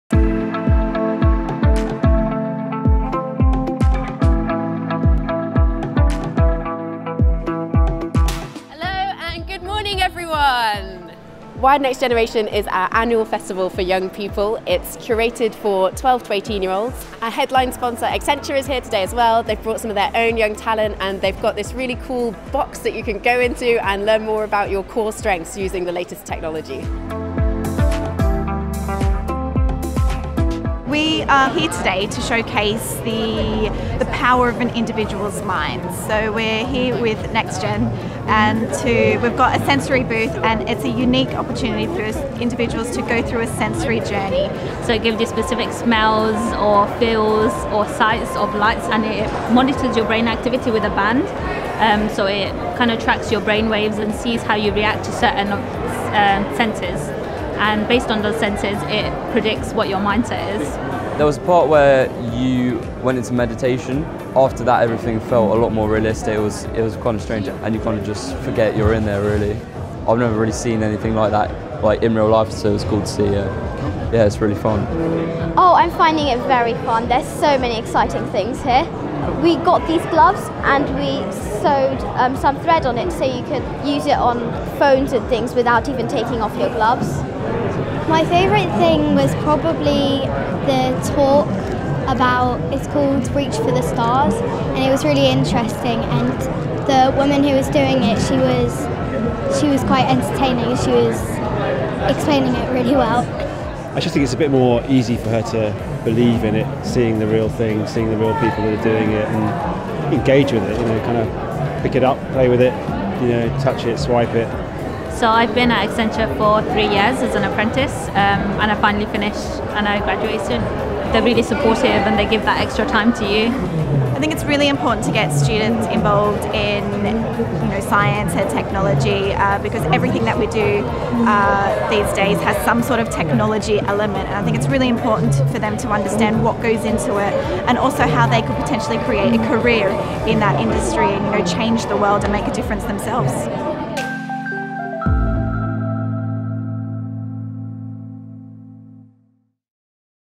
Wired Next Generation features MyndPlay and Accenture
Wired Next Generation is an educational and interactive event for young people aged 12-18 interested in business, science and technology. This time it featured the booth we created for Accenture Powerful Minds campaign